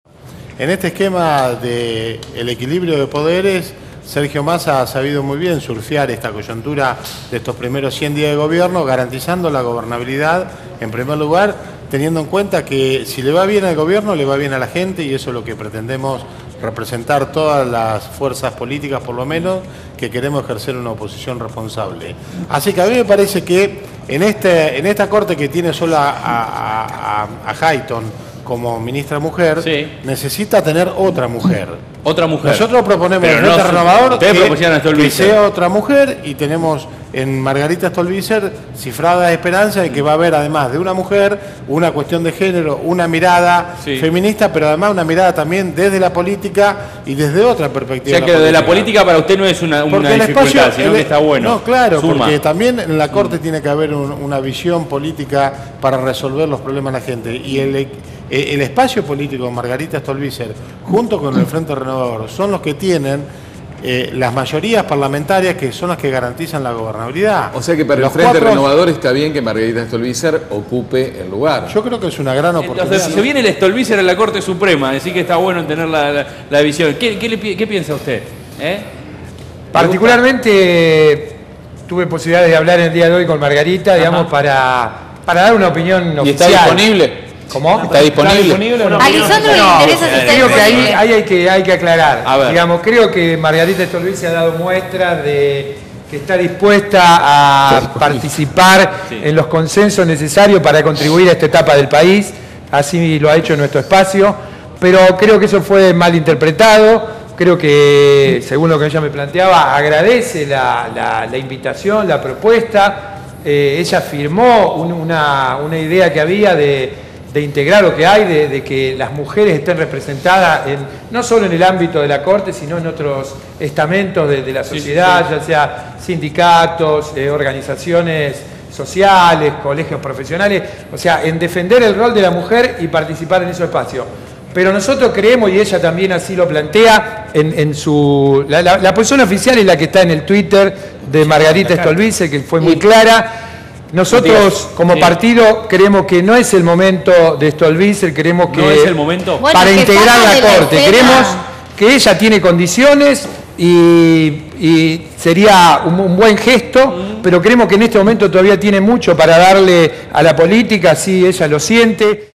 DEMASIADO HUMO - ¿MARGARITA STOLBIZER A LA CORTE SUPREMA?
RICARDO LISSALDE, DIP. PROV. FRENTE RENOVADOR
JUAN COCINO, DIP. PROV. GEN